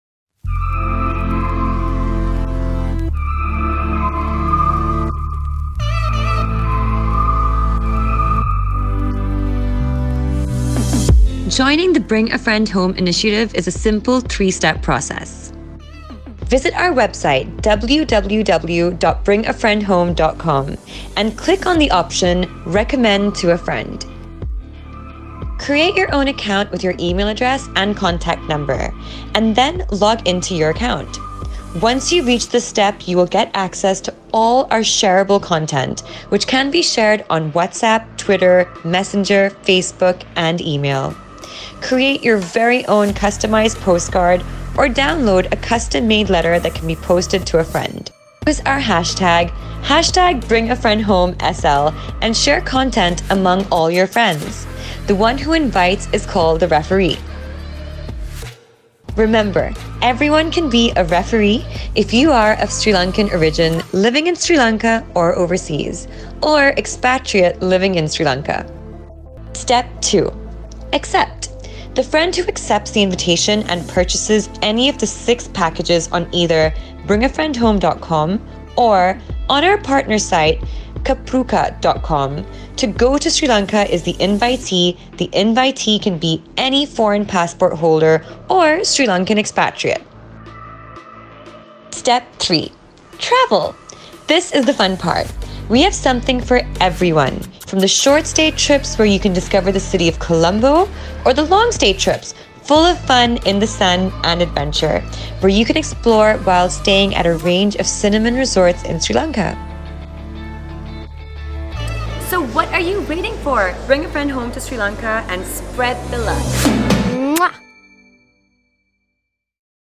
Jacqueline Fernandez I Bring a Friend Home to Sri Lanka
Bollywood star Jacqueline Fernandez shares how you can be a brand ambassador to Sri Lanka by encouraging tourism back to our island home!

To be a part of our Bring a Friend Home initiative, all you need to do is invite your friends to Sri Lanka for great discounted packages and win amazing prizes for you and your friends.

Here is a step by step process showing you how you can come on board.

Don’t forget to use the hashtag BringaFriendHomeSL and spread this message far and wide!